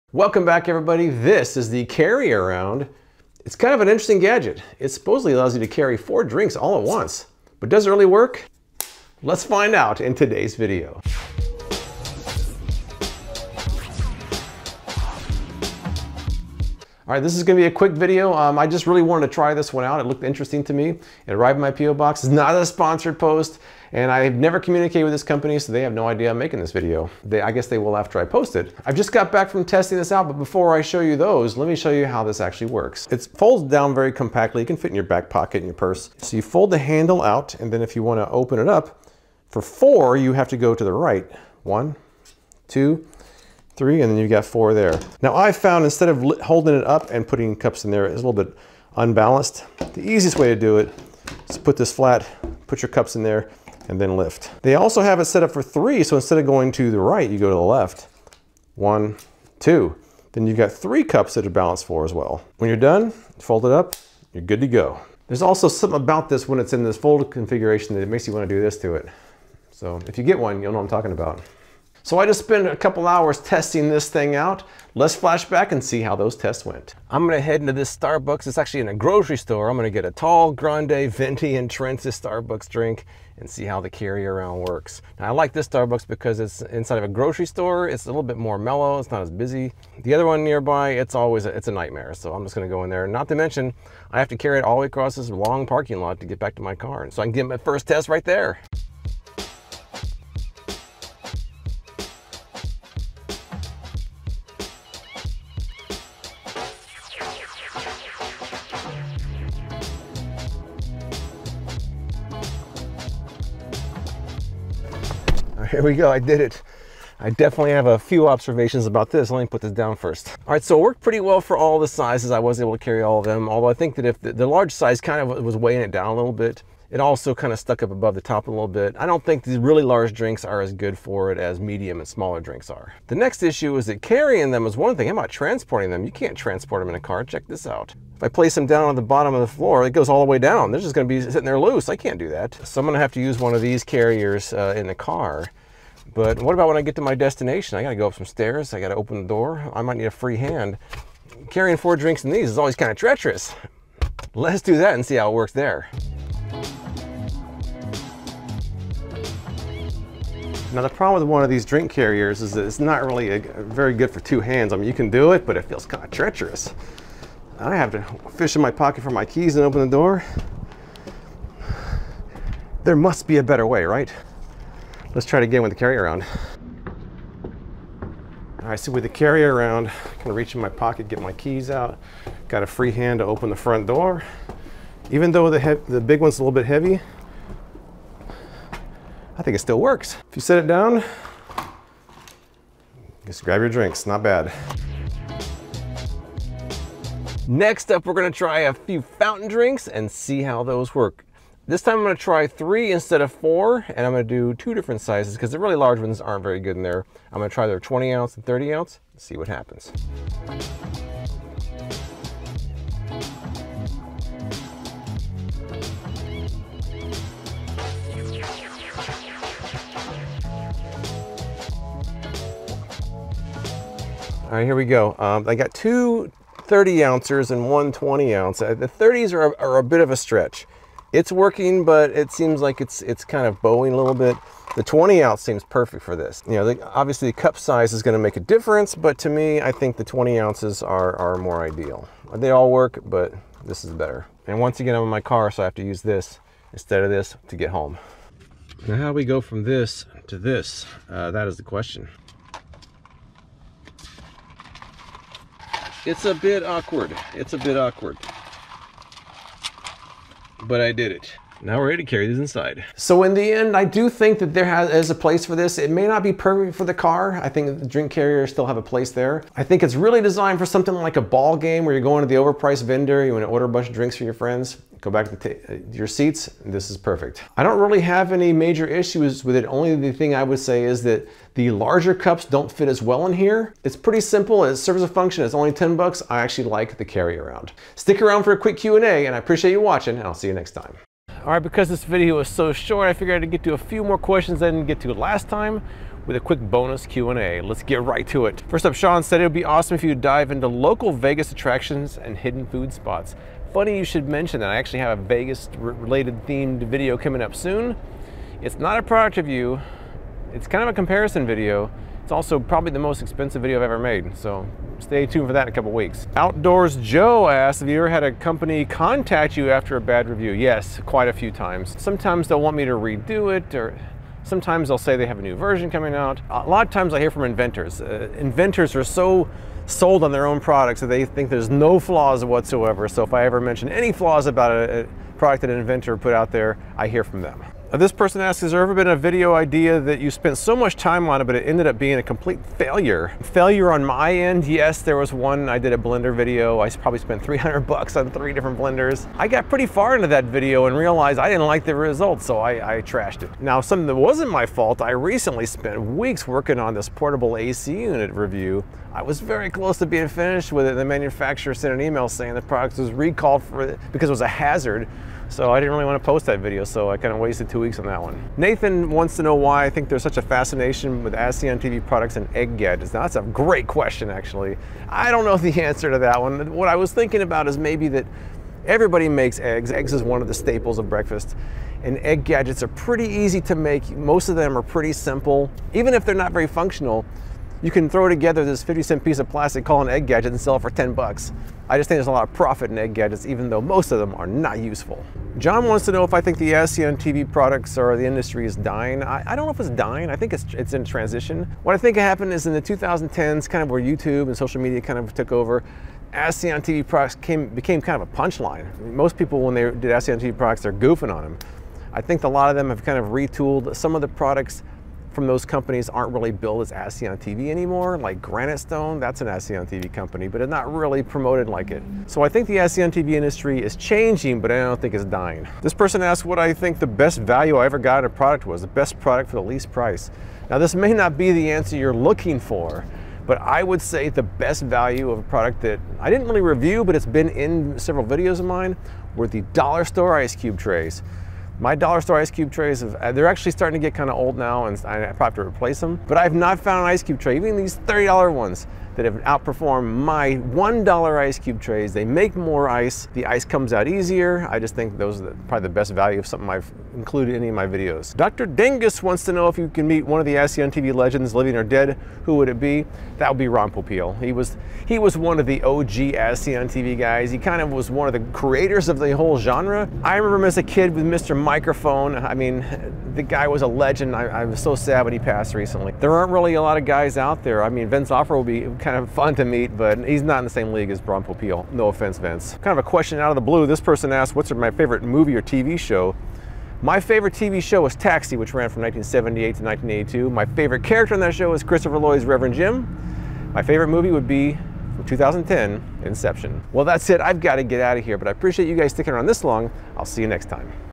This $10 Drink Gadget is Pretty Clever - plus Q&A
It's a drink carrier that can allow you to carry up to 4 drinks with one hand.

GET STUFF
• Freakin Reviews Gadget Store

GEAR
• Main Camera
• Main Lens
• 2nd Camera

0:00 Intro & Overview
1:12 4-Cup Test
3:15 3-Cup Test
4:22 Conclusion
4:51 Q&A